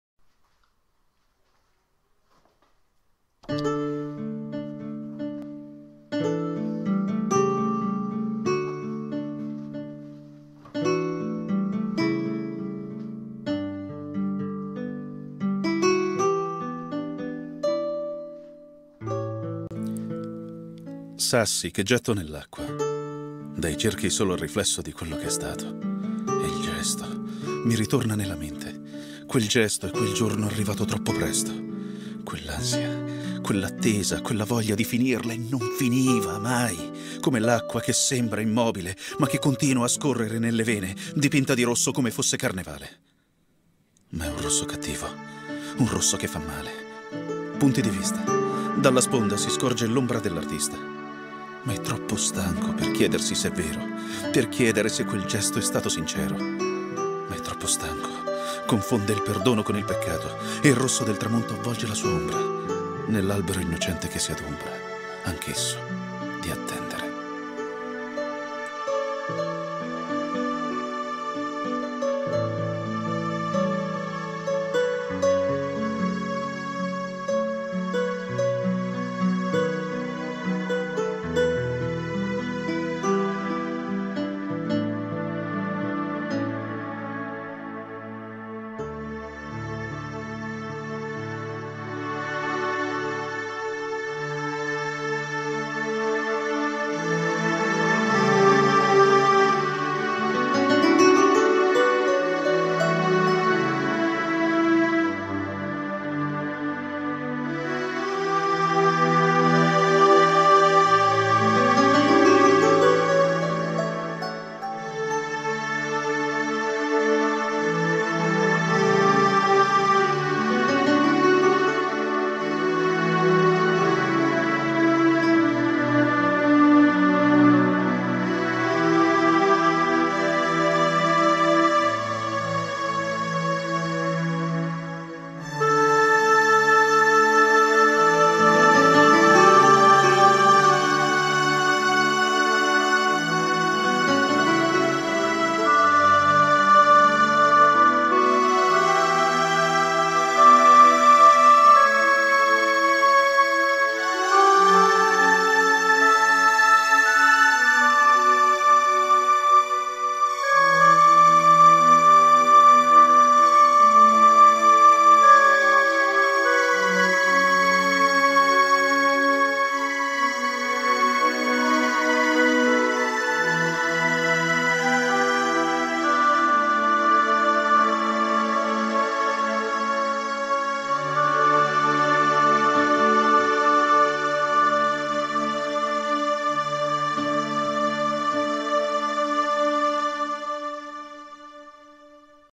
L'artista sulla riva
L'artista sulla riva  _ poesia scritta da Franco Marnati
recitata da Diego Baldoin su musica originale di Alessandro Benanti